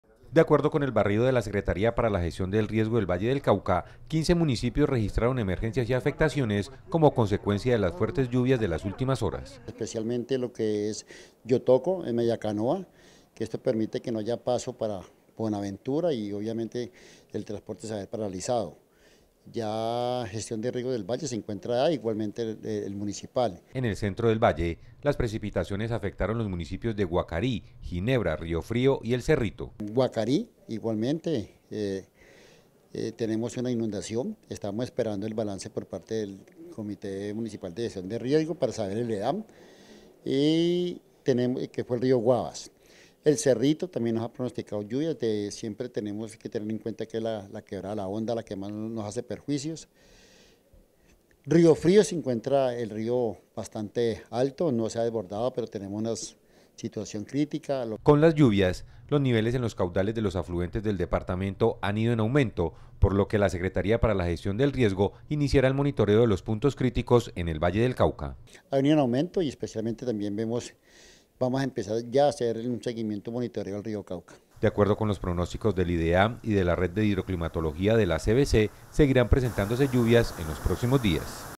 15 municipios del Valle del Cauca registraron afectaciones y emergencias por fuertes lluvias
Emitido el 6 de Abril de 2018 a las 9 p.m.  por el Canal regional Telepacífico.